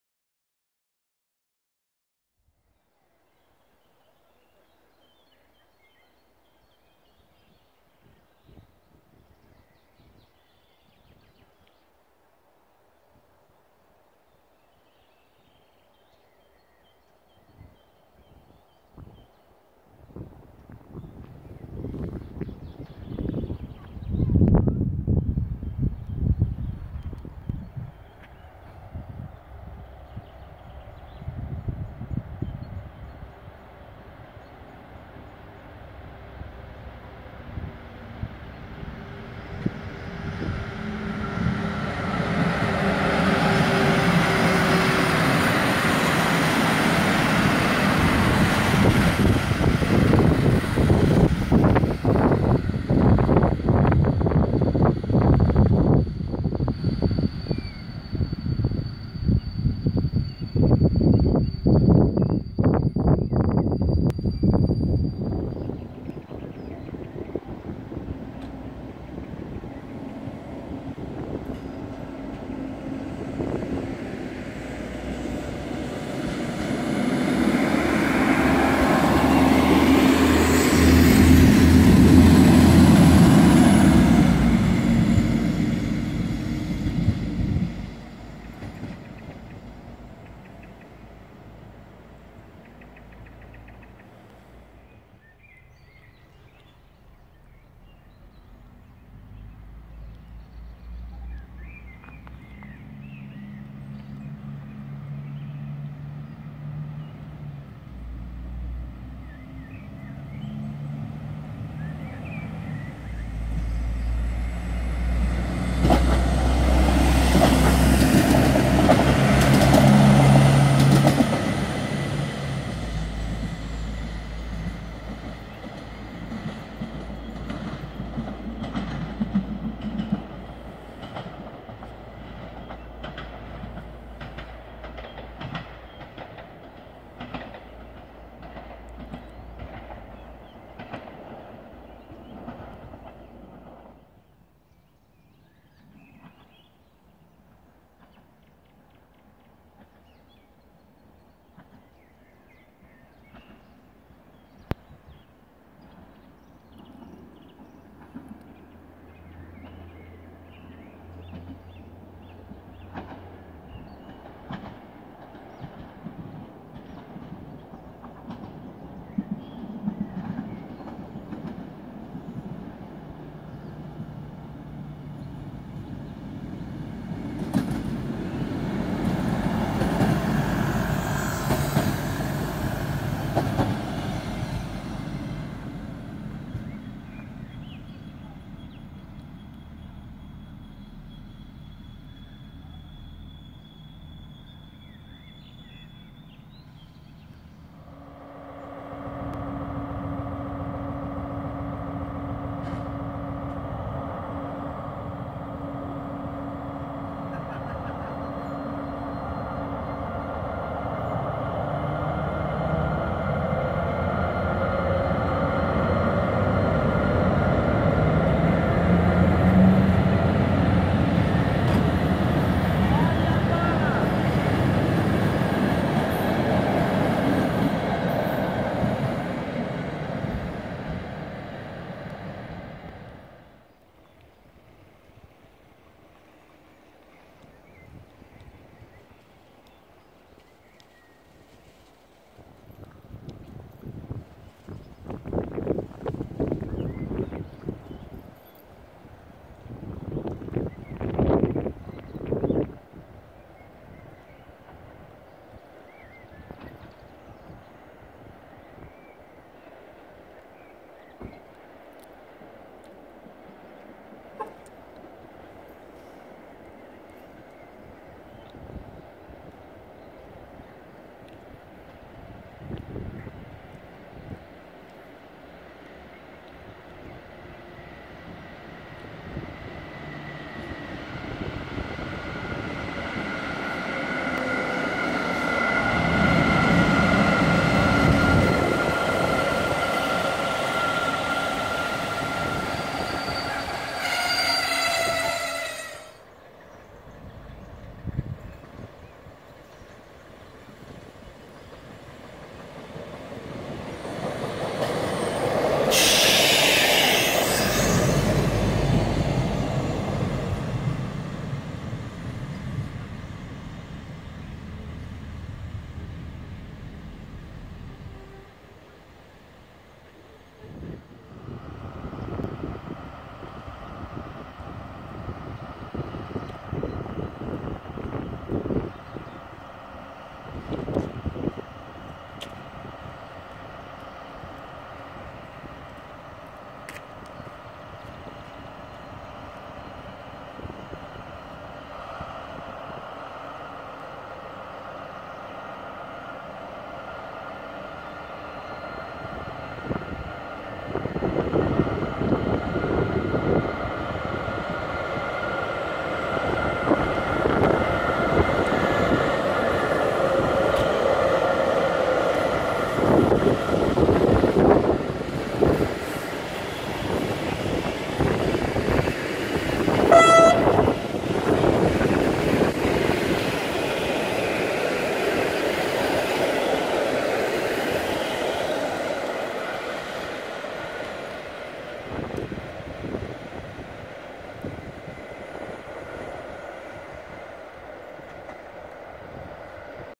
Kolej w Kraśniku
Nowa, rozszerzona wersja filmu uwzględniająca wszystkie pociągi, jakie widziałem tam w ciągu dwóch dni mojej pracy w Kraśniku :)
W krainie lubelskich pociągów spalinowych...
Pociągi osobowe relacji: Lublin - Stalowa Wola Rozwadów i TLKi Staszic relacji: Rzeszów Główny - Piła Główna i na odwrót. Dodatkowo mała drezynka i skład, z którego zrzucaliśmy szyny na szlaku Wilkołaz - Kraśnik.